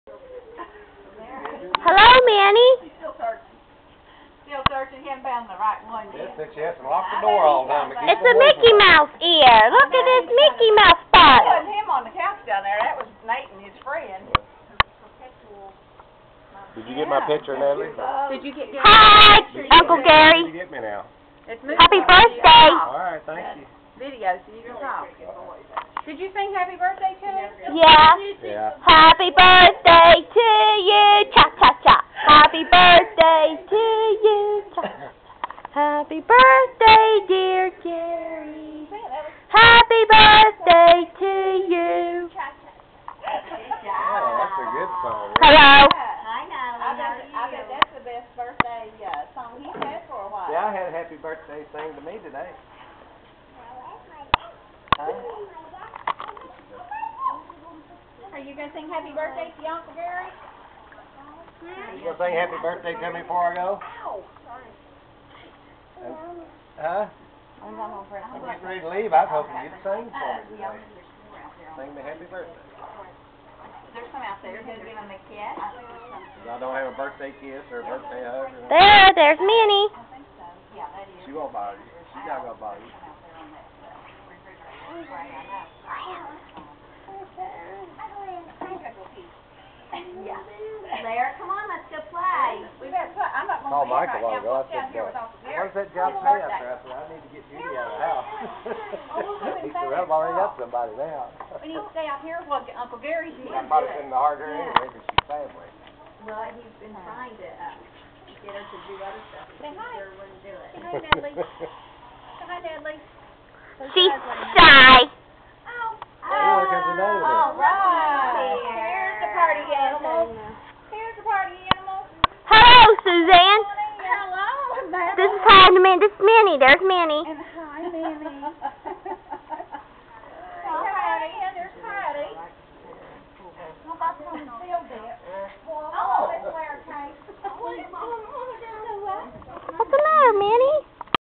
My Uncle Gary's Birthday 1
His Dog Minnie She is going with her daddy
R.I.P  Minnie Curtis Born 1/20/1996 Died 12/1/2009